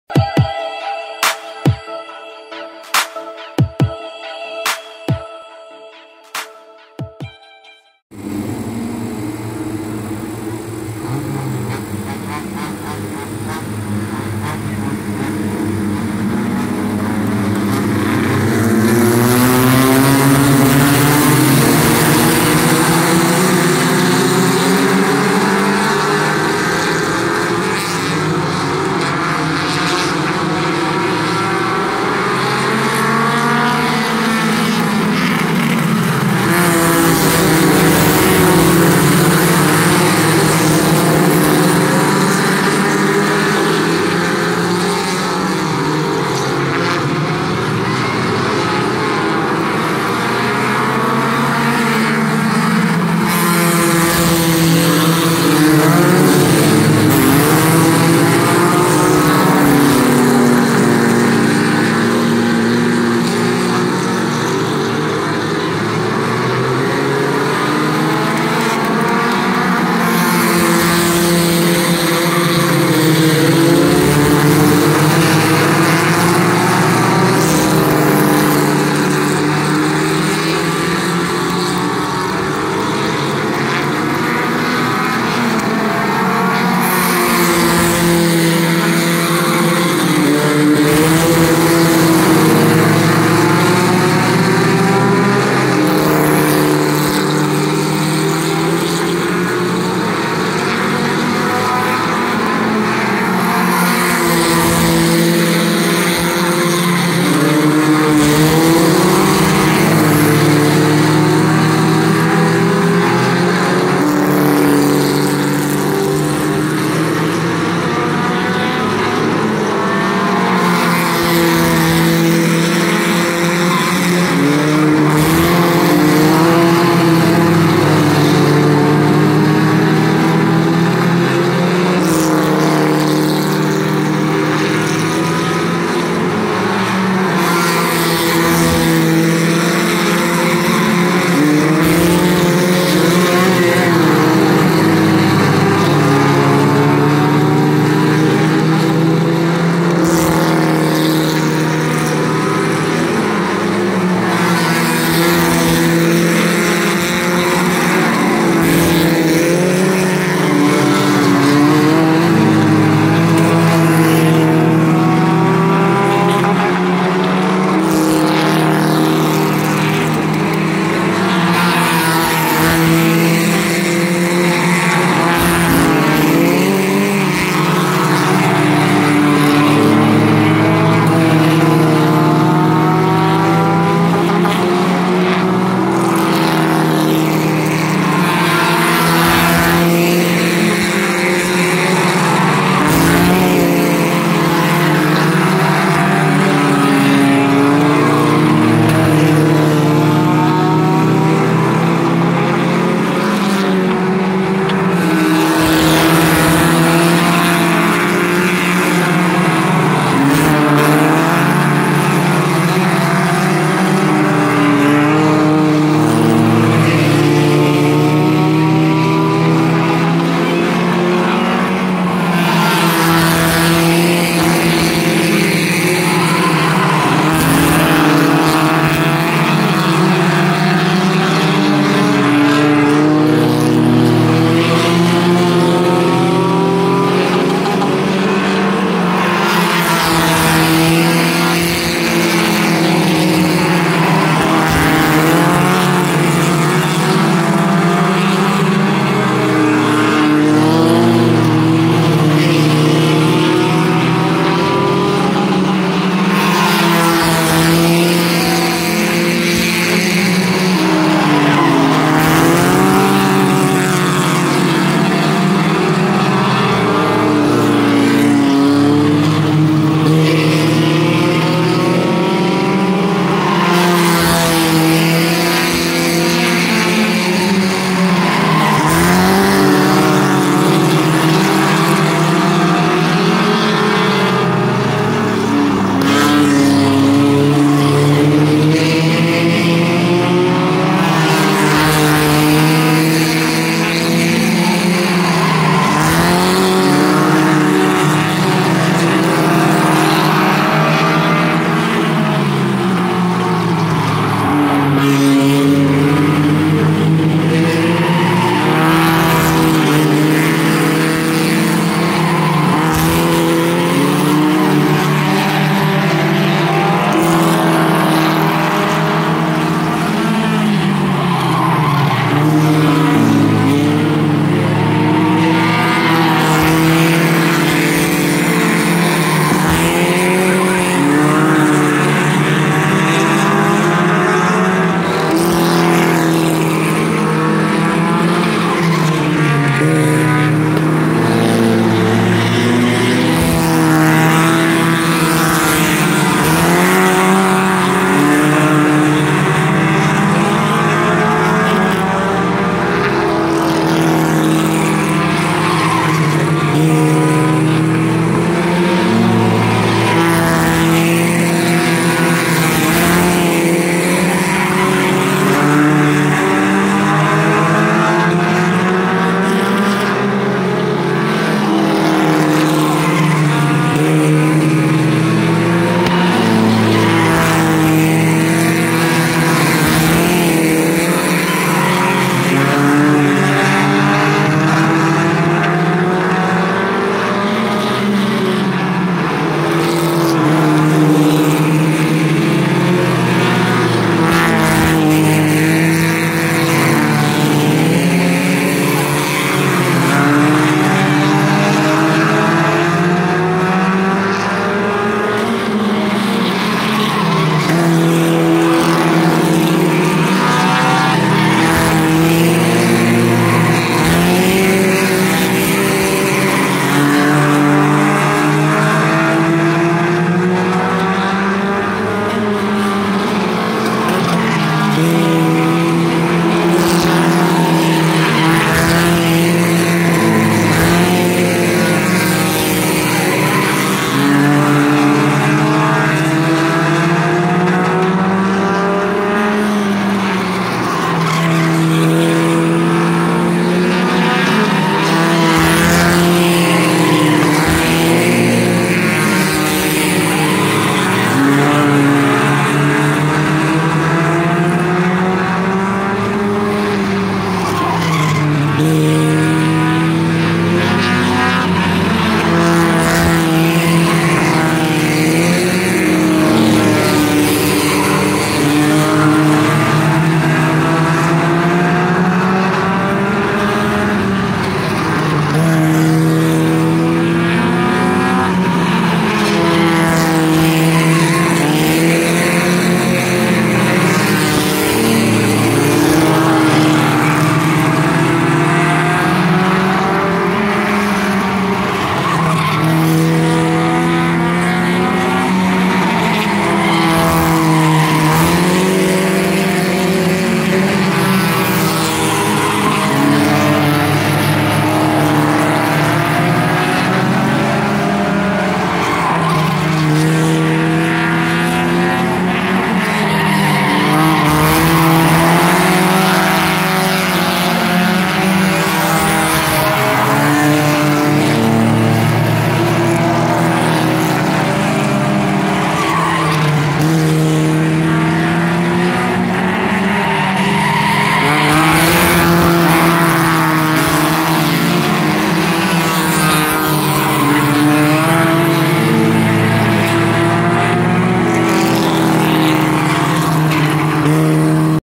Auburndale Speedway Scrambler Feature #1
Night 1 of the charity race weekend. First 30 lap feature of two.